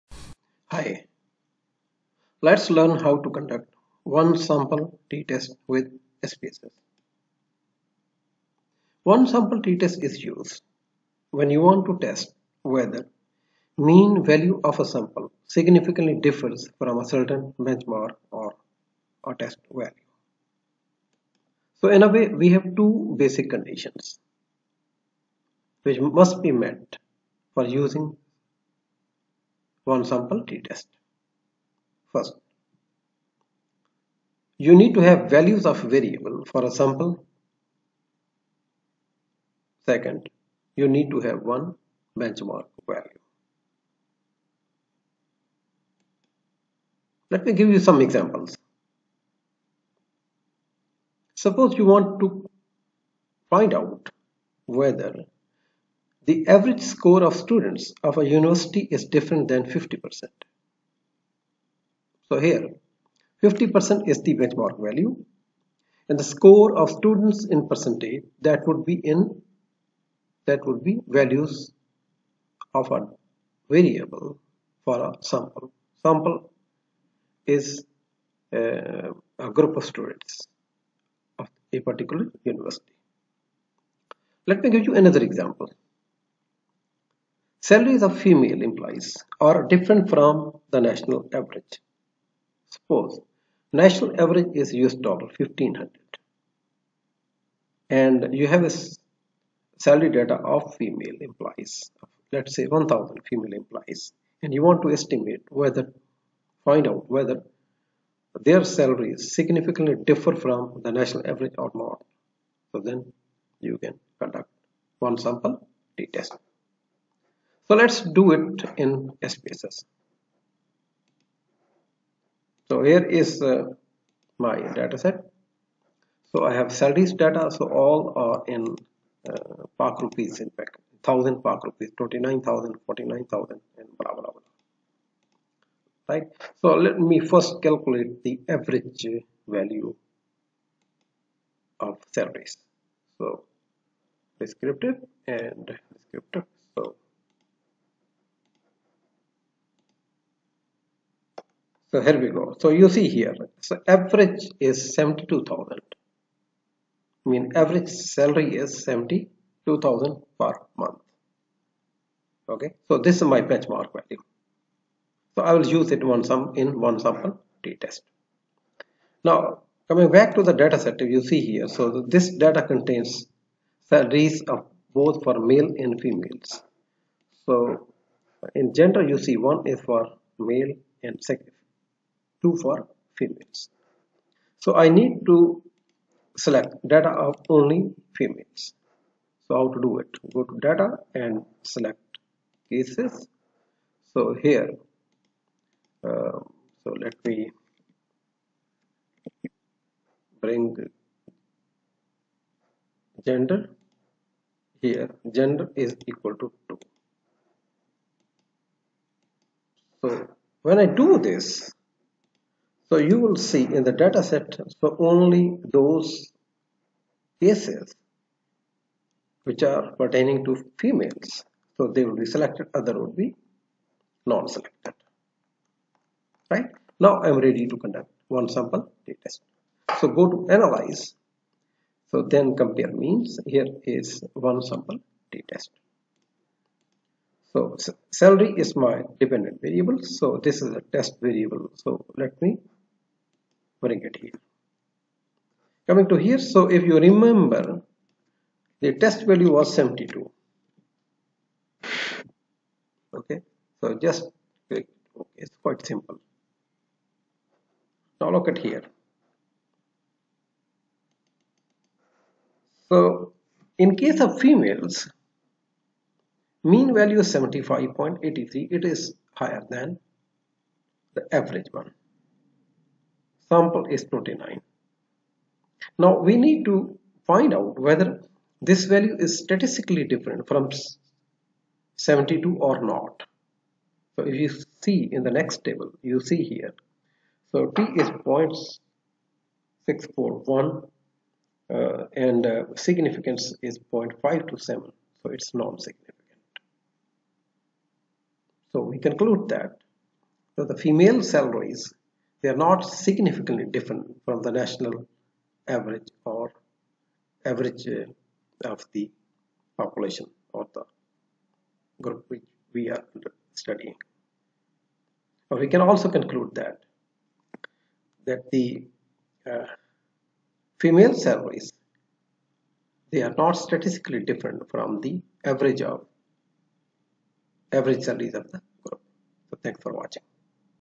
One Sample T-test with SPSS
Learn when and how to conduct one sample t-test with SPSS